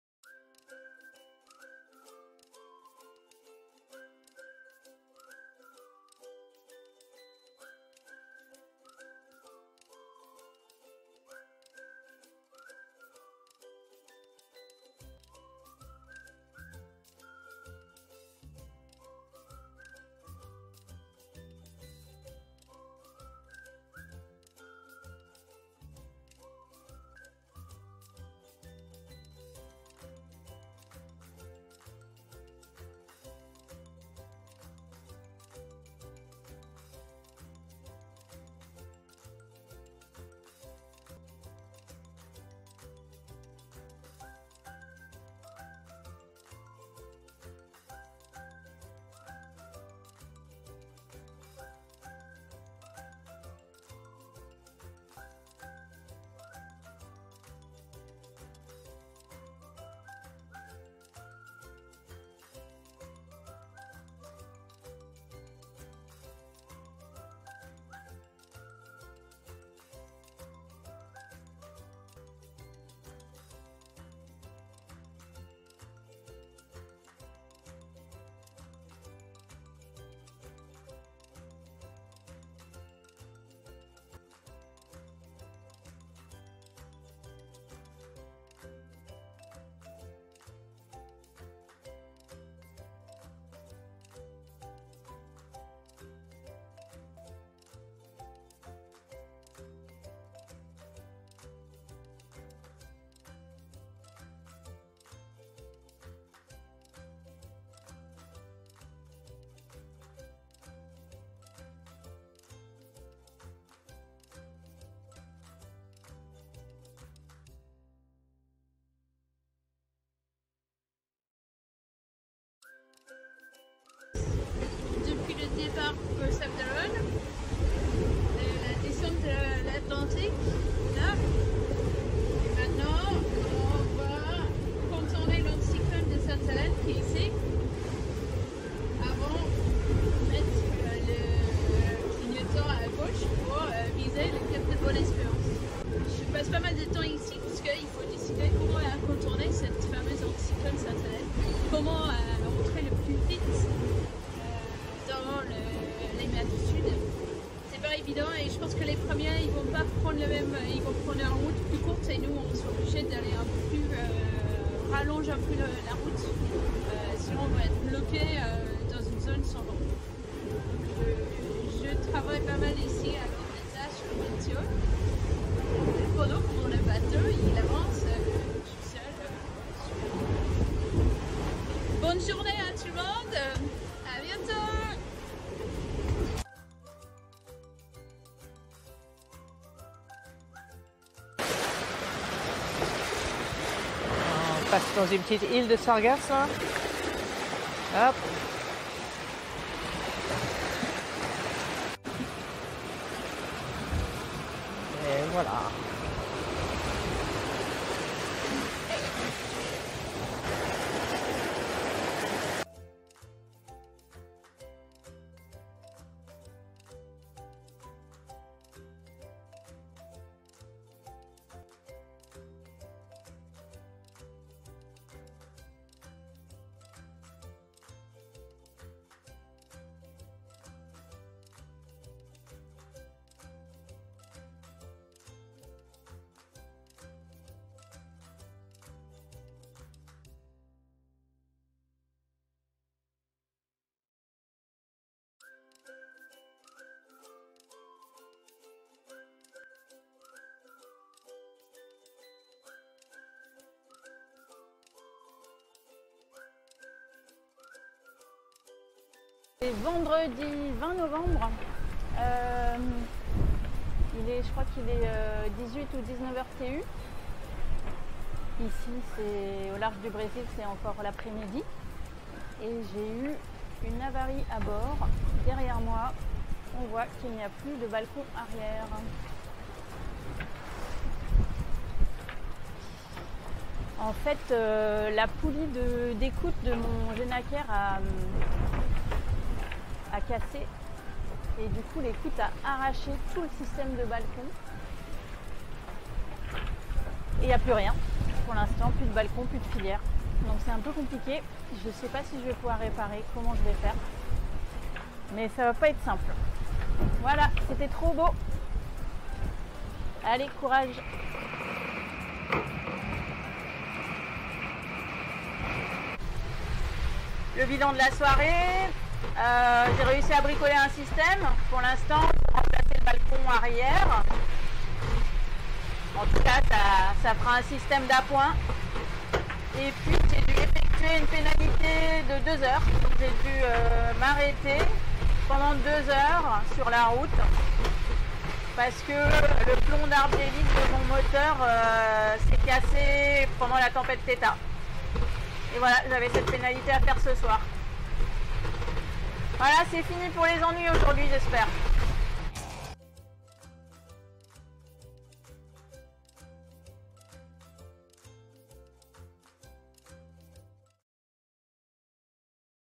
Vendée Globe 2020 Jour 14 et 15 pour les enfants
JOUR 14 ET JOUR 15
Chers enfants,
Comment s'est passé votre we ? Voici la vidéo récapitulative de tout ce qui s'est passé en mer ce we ! Bonne vidéo !

Images de google, des comptes Instagram et Facebook des skippers et de l'organisation du vendée globe.